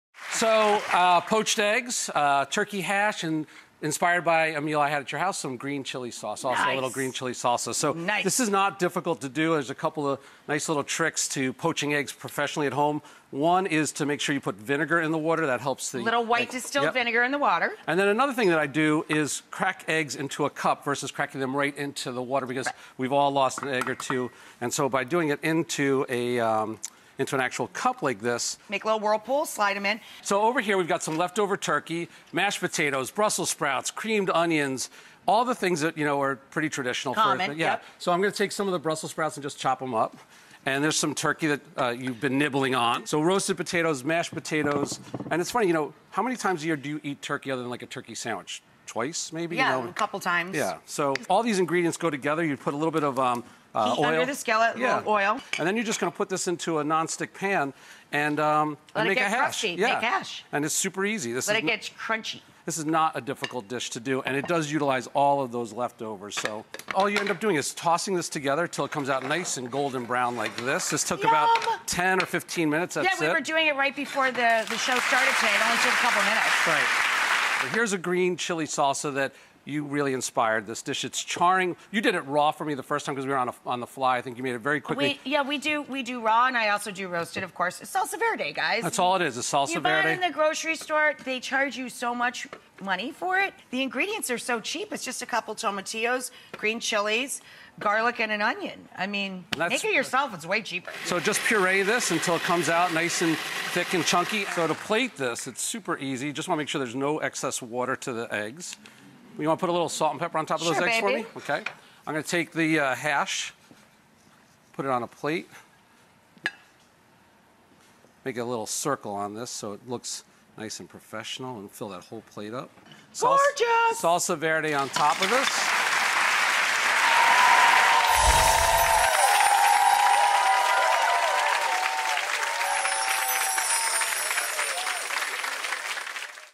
How to Make Spicy Turkey Hash with Poached Eggs & Green Chile Sauce by Michael Schlow
With poached eggs and a spicy green chile sauce!